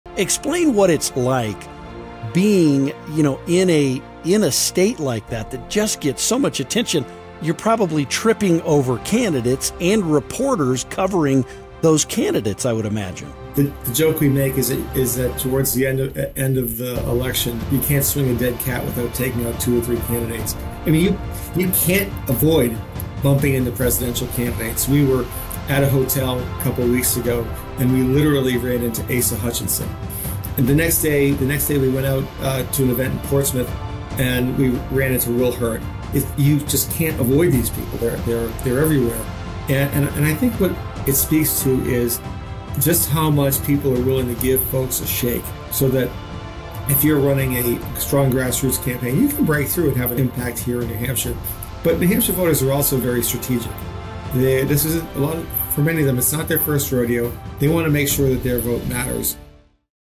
Inside the Primary: New Hampshire's Decisive Dance with Democracy
Journey into the heart of New Hampshire's vibrant political scene with host Jeff Crank and special guest Greg Moore, State Director for Americans for Prosperity in New Hampshire. Together, they dissect the state's pivotal role in the presidential primaries, delve into the art of grassroots campaigning, and spotlight the discerning New Hampshire voters who often set the tone for the nation. Experience firsthand the passion, strategy, and authenticity that define the Granite State's unique dance with democracy.